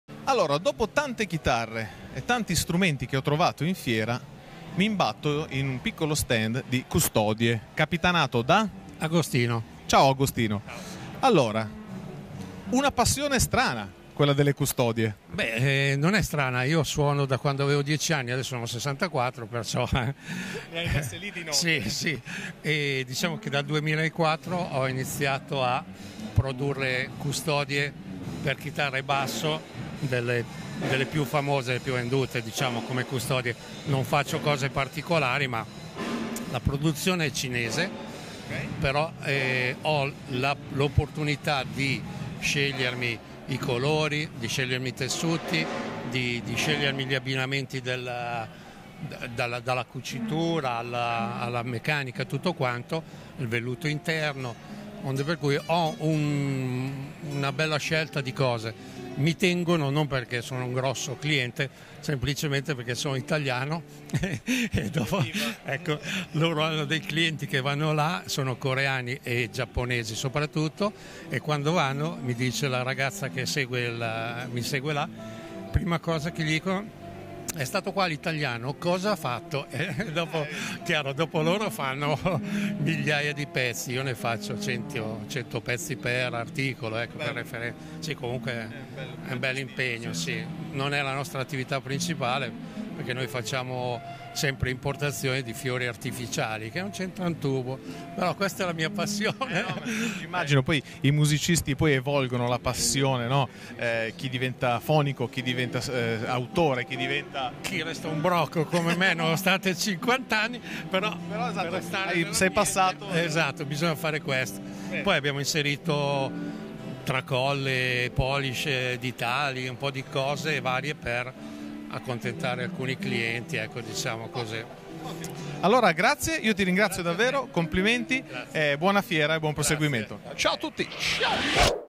SHG Music Show 2019: Aquattro Service
VideoLive ha intercettato per noi le novità più calde di SHG Music Show Milano 2019.
Continua a guardare la playlist completa nell'articolo su Accordo